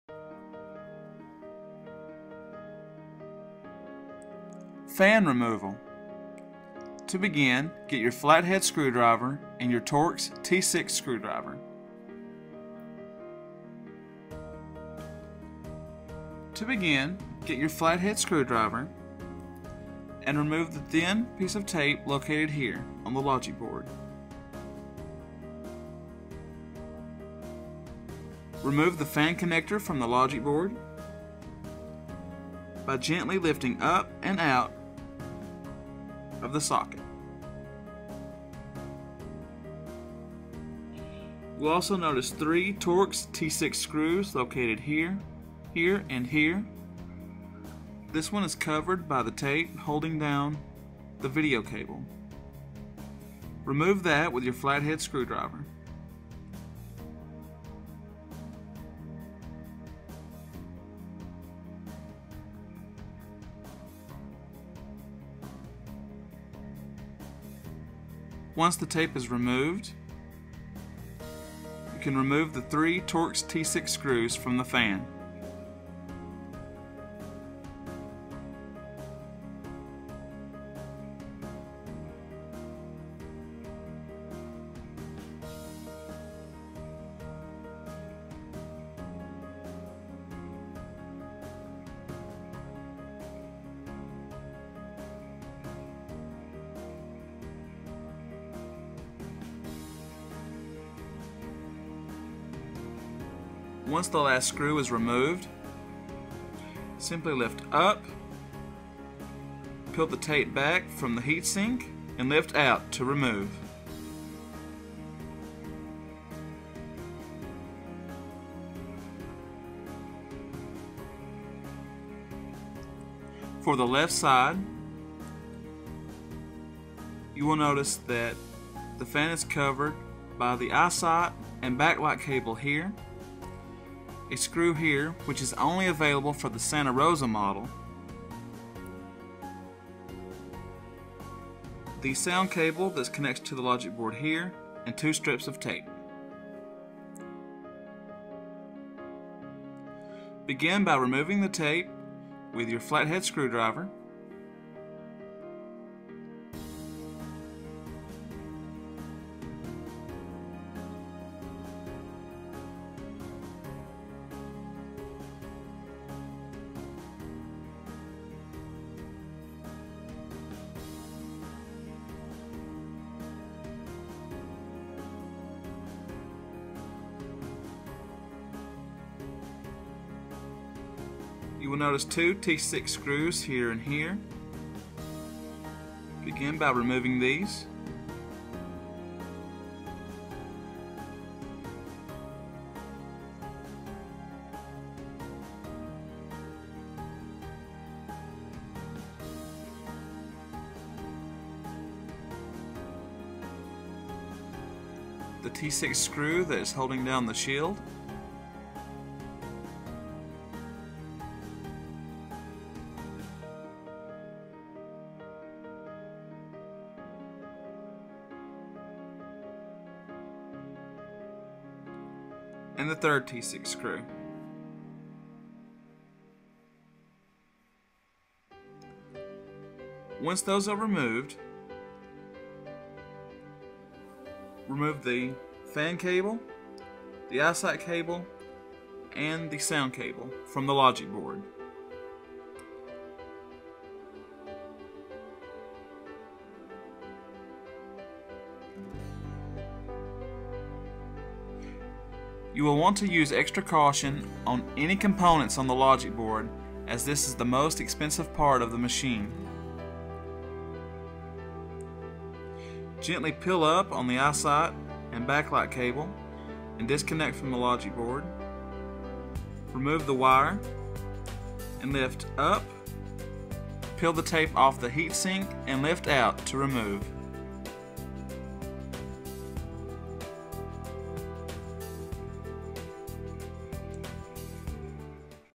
Macbook Pro 15" Repair - Fan Removal
A guide on how to remove the fans in a 15" Macbook Pro. for more repair guides.